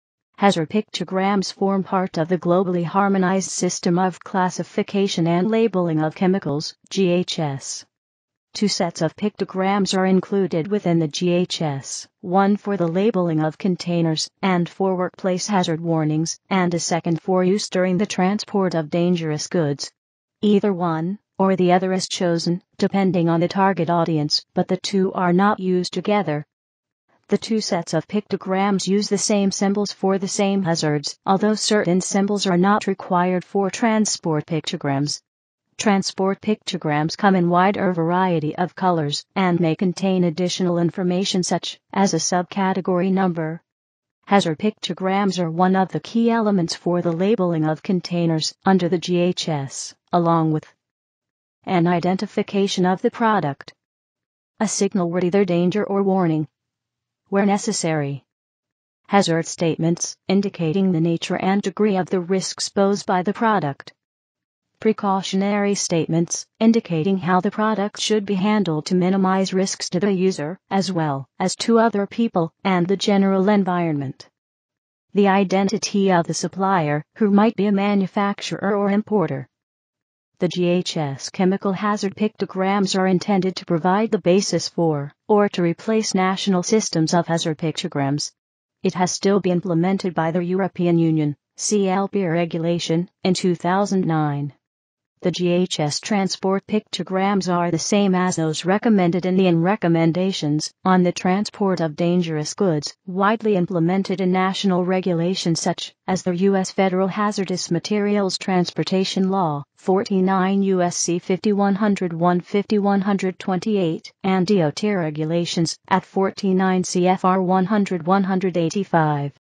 GHS hazard pictograms
Hazard pictograms form part of the Globally Harmonized System of Classification
and Labeling of Chemicals (GHS). Two sets of pictograms are included within the
GHS: one for the labeling of containers and for workplace hazard warnings, and a
second for use during the transport of dangerous goods. Either one or the other
is chosen, depending on the target audience, but the two are not used together.
The two sets of pictograms use the same symbols for the same hazards, although
certain symbols are not required for transport pictograms. Transport pictograms
come in wider variety of colors and may contain additional information such as a
subcategory number.